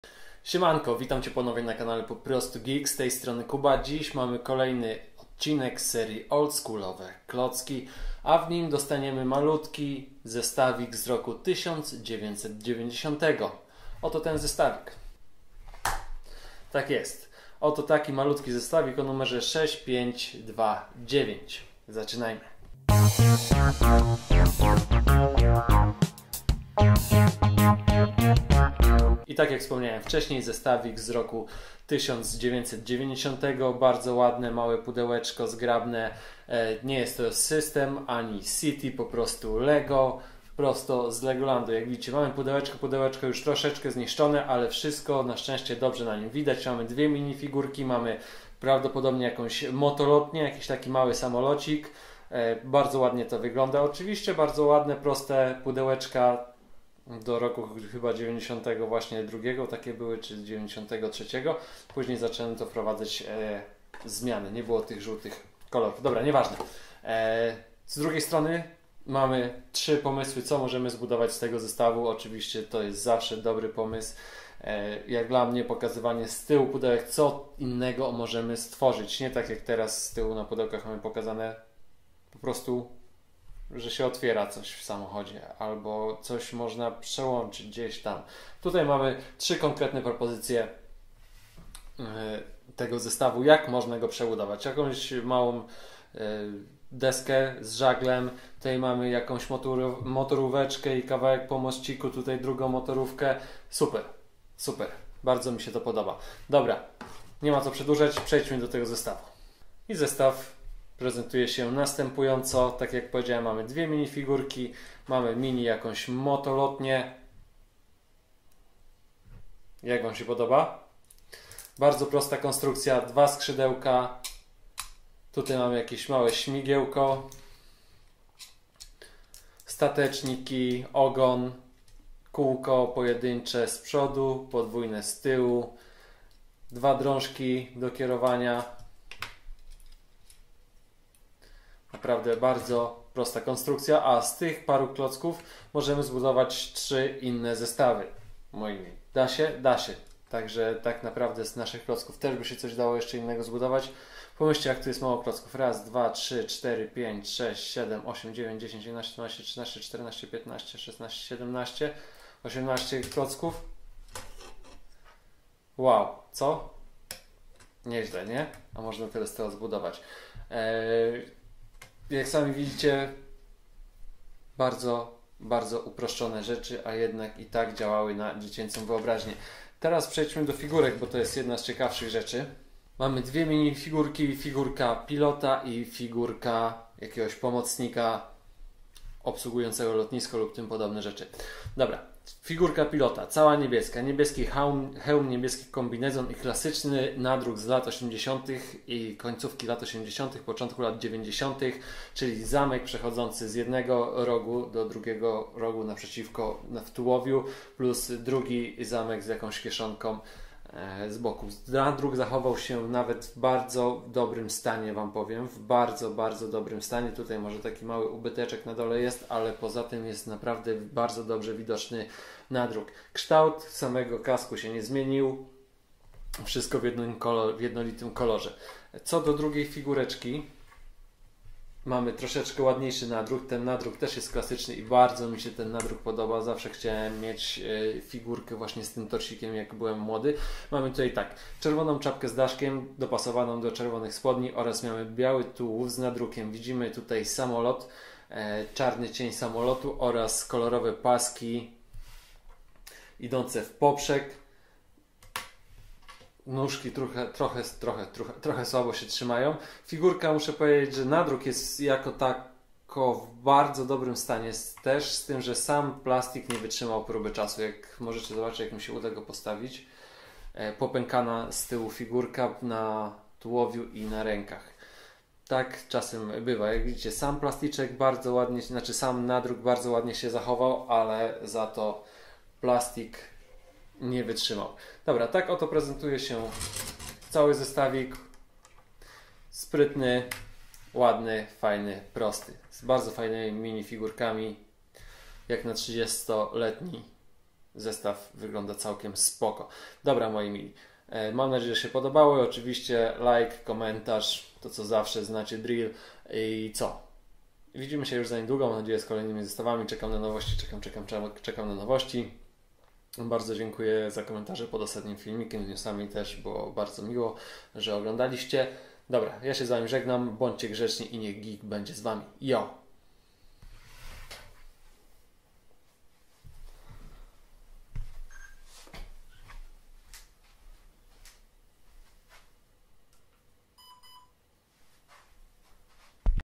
OldSCHOOLovE Klocki LEGO 1990 ROK 6529 recenzja
Zapraszam na recenzję zestawu Lego 6529 z roku 1990